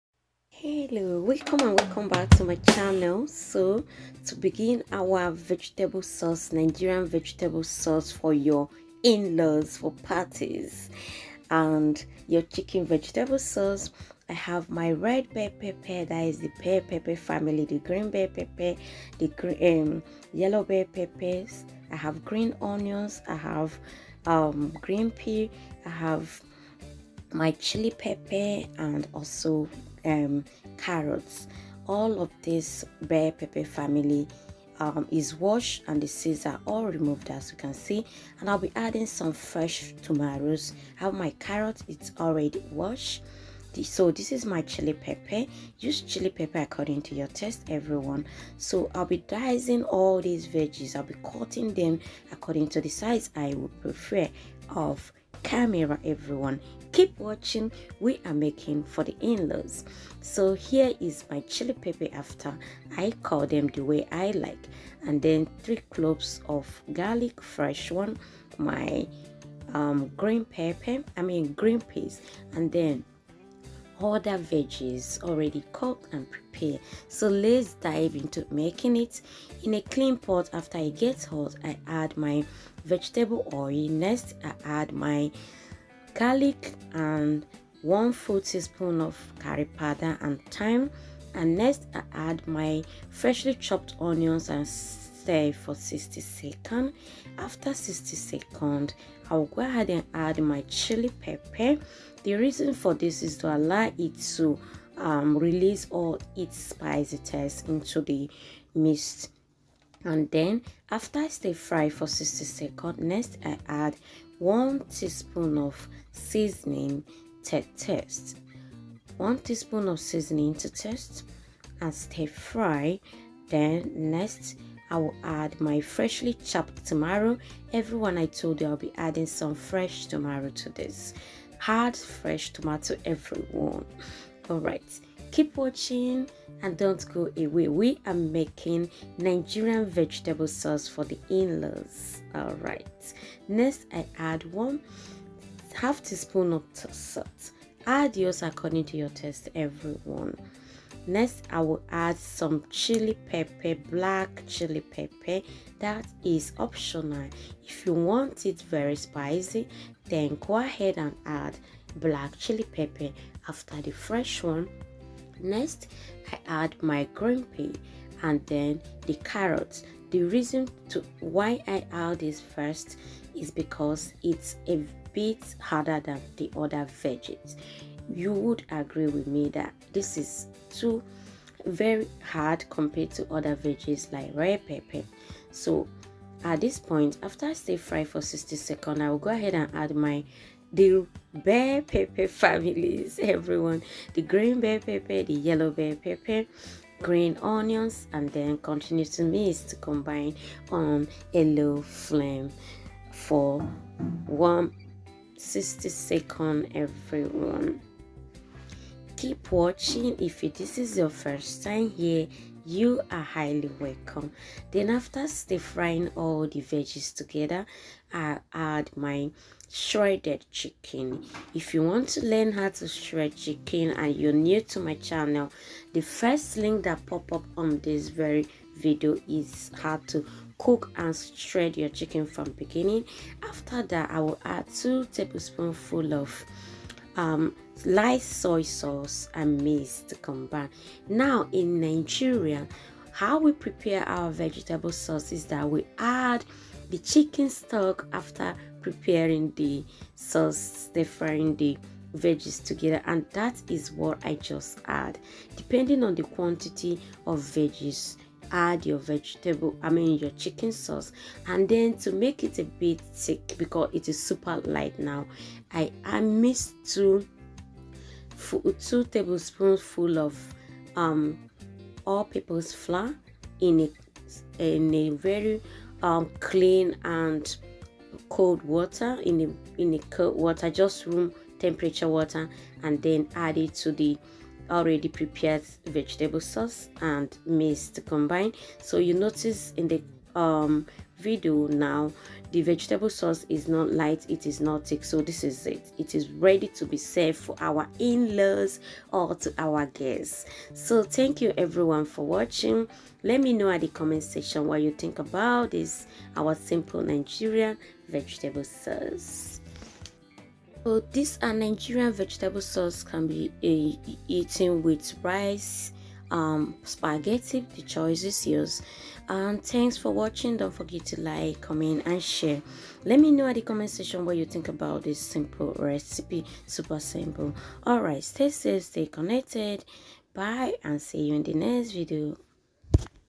How To Make Chicken Vegetable Soup For Nigeria inlaws.
In this video, I share with you how to prepare simple vegetable Soup for Nigerian inlaws, delicious bur very easy to make.

2. how to cook and shredd chicken

3. Nigerian soup recipe